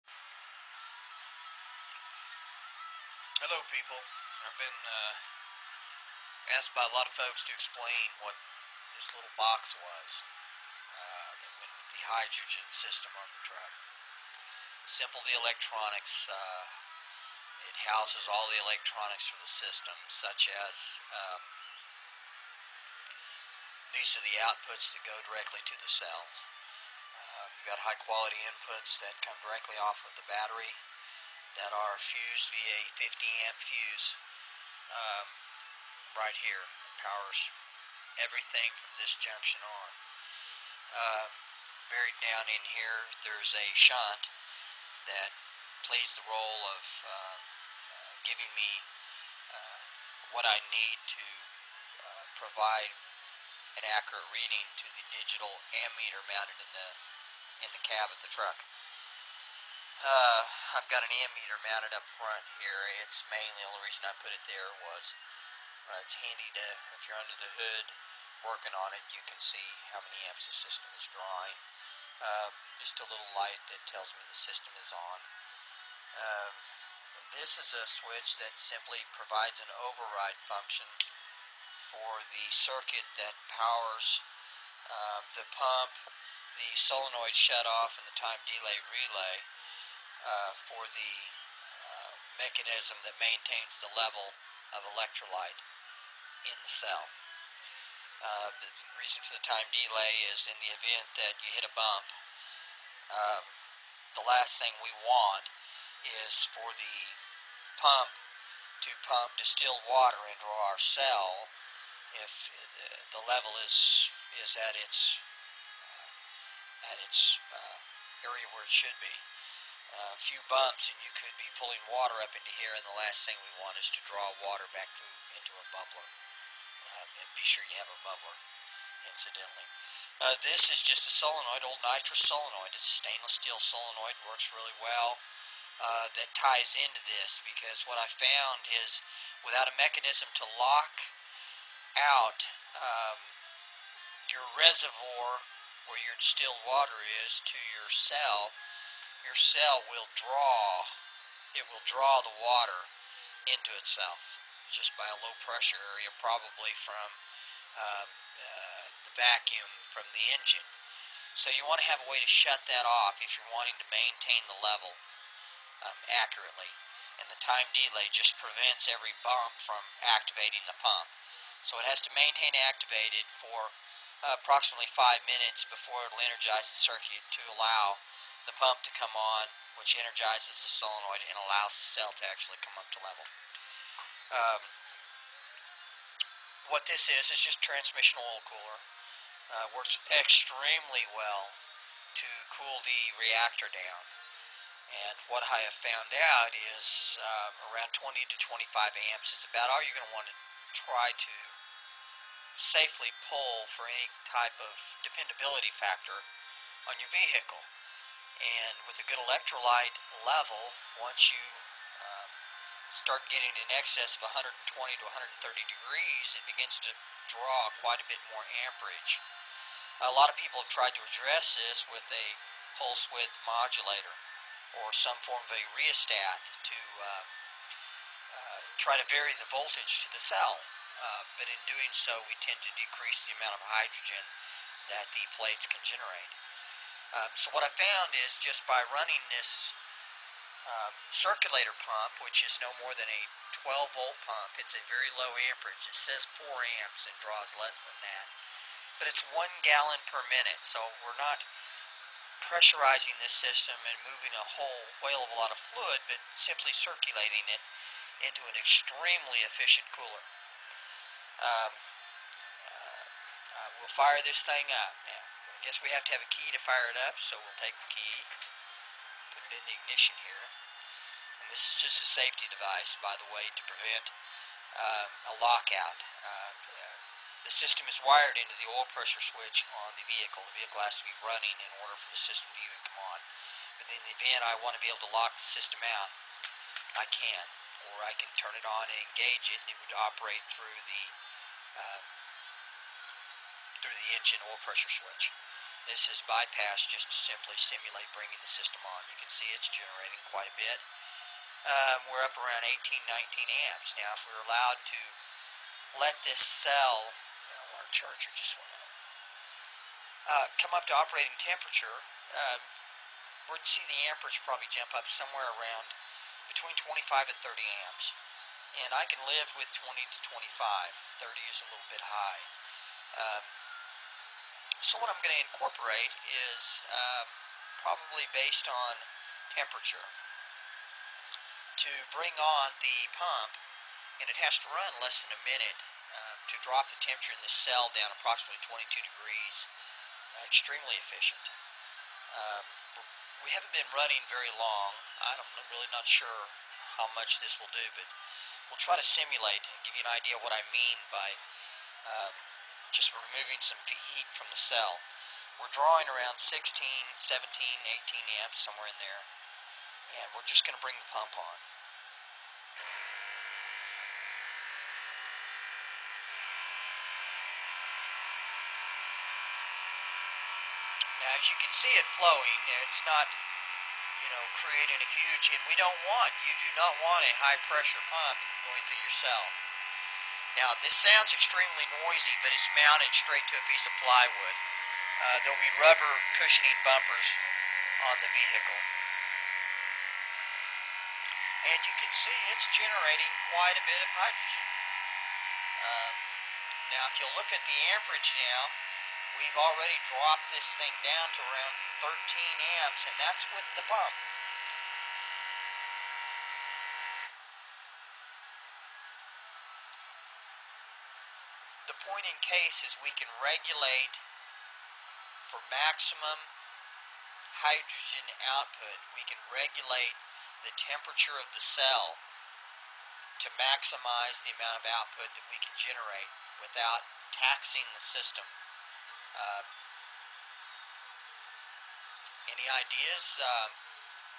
Hydrogen Generator
Maintain HHO Production By Regulating Temp of Reactor Cell